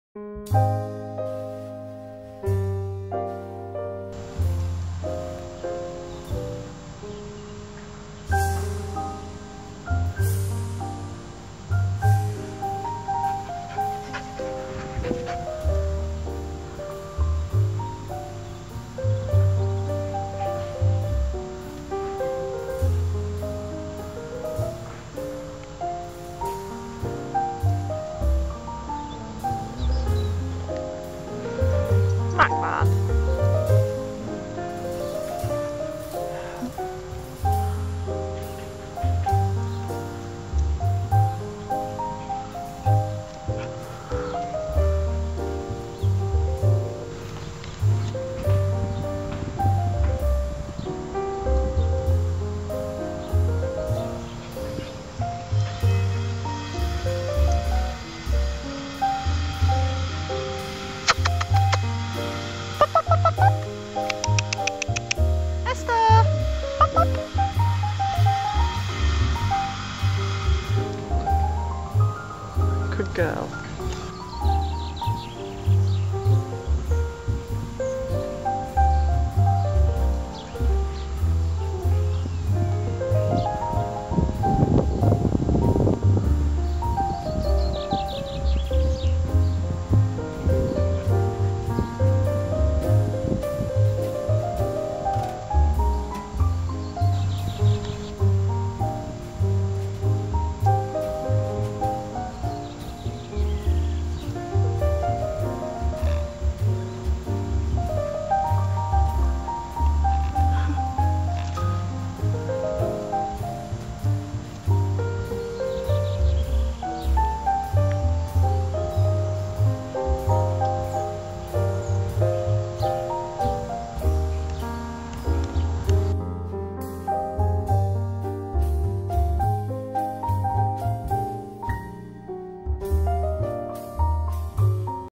esther - 10 weeks old.wmv
Esther - Bryning Bleak House.  10 week old parson russell terrier pup.  CH Giftrapped of Lovealoch to Pacolito x Glendream Glad all over at Bryning.